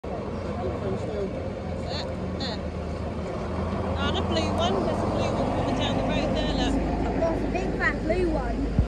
AEC Renown 3B2RA | 596LCG / 2212 | Route 18 | Ex King Alfred | Winchester Broadway | 06/05/24
A 1964-built AEC Renown 3B2RA with a Park Royal H44/31F body formerly operated by King Alfred painted in the National Bus Company (NBC) poppy red livery with the Hants & Dorset logo owned by the Friends of King Alfred Buses (FoKAB) terminates at Winchester Broadway on the route 18 service from Springvale during the Original King Alfred Running Day.